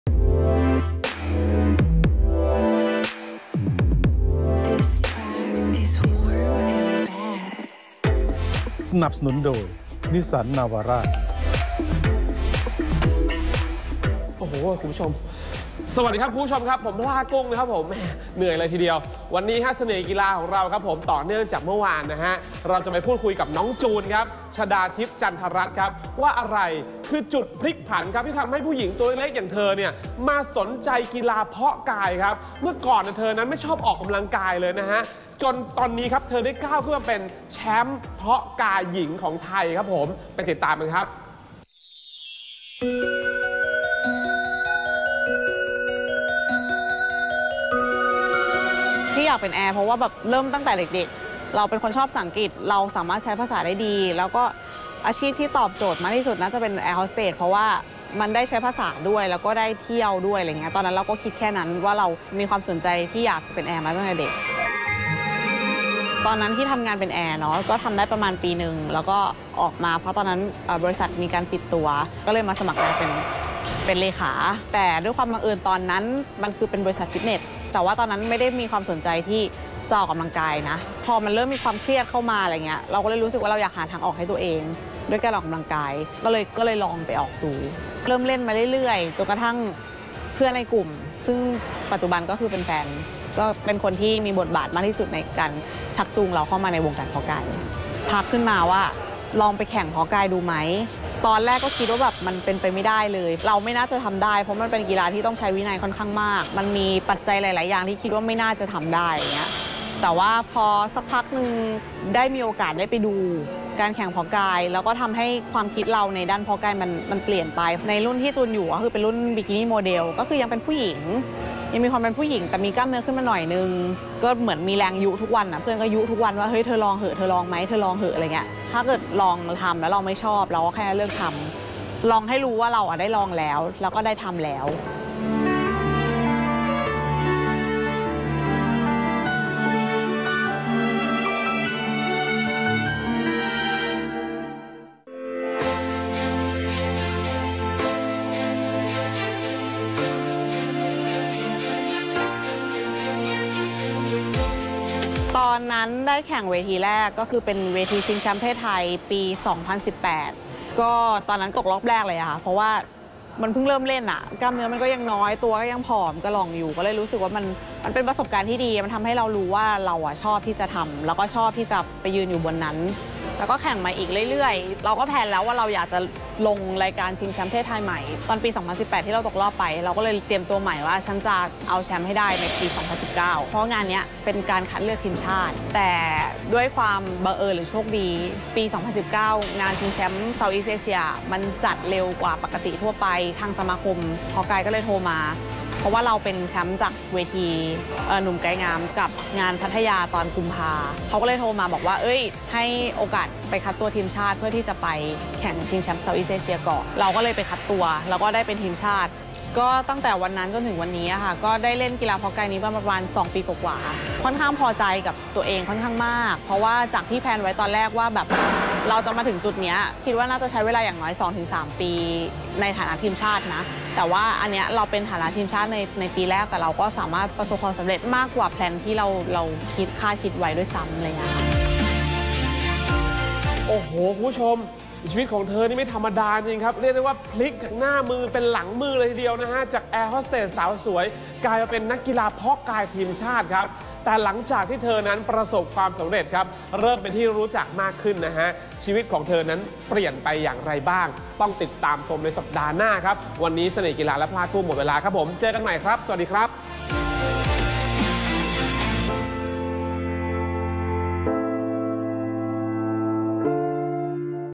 จุดพลิกผันของ ‘จูน ชฎาทิพย์’ จากแอร์โฮสเตส สู่ นักเพาะกายหญิงทีมชาติไทย
พูดคุยกันต่อกับสาวแกร่ง ‘จูน’ ชฎาทิพย์ จันทรัตน์ นักกีฬาเพาะกายทีมชาติไทย ว่าอะไรคือจุดพลิกผันที่ทำ จูน มาสนใจในกีฬาเพาะกาย ทั้งที่เมื่อก่อนเธอไม่ค่อยชอบออกกำลังกายด้วยซ้ำ

‘จูน’ เริ่มต้นการทำงานด้วยอาชีพแอร์โฮสเตส เพราะตอนเด็กๆ ชอบภาษาอังกฤษและใช้ภาษาได้ดี ซึ่งอาชีพที่ตอบโจทย์คือแอร์โฮสเตส ทำได้ประมาณ 1 ปี บริษัทได้ปิดตัวลง จึงได้มาสมัครเป็นเลขานุการ ในบริษัทฟิตเนสแห่งหนึ่ง ซึ่งจูนบอกว่าตอนนั้นไม่ได้มีความสนใจเรื่องการออกกำลังกายเลย พอถึงจุดหนึ่งเริ่มมีความเครียดเข้ามา จึงหาทางออกให้ตัวเองด้วยการออกกำลังกาย